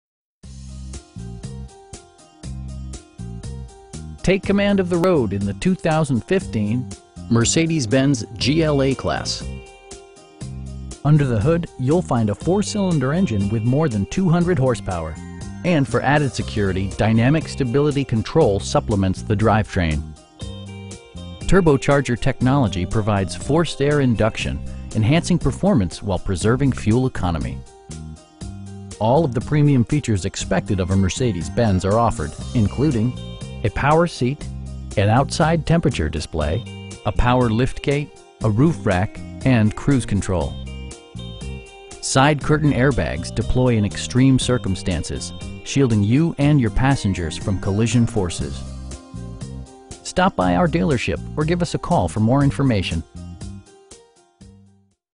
2015 Mercedes-Benz GLA-Class 4matic 4dr GLA250
Bergstrom Enterprise Motorcars
3002 Victory Ln

Take command of the road in the 2015 Mercedes-Benz GLA-Class. Under the hood you'll find a 4 cylinder engine with more than 200 horsepower, and for added security, dynamic Stability Control supplements the drivetrain. Turbocharger technology provides forced air induction, enhancing performance while preserving fuel economy. All of the premium features expected of a Mercedes-Benz are offered, including: a power seat, an outside temperature display, tilt and telescoping steering wheel, a power liftgate, power windows, a roof rack, and cruise control. Side curtain airbags deploy in extreme circumstances, shielding you and your passengers from collision forces. We pride ourselves on providing excellent customer service. Stop by our dealership or give us a call for more information.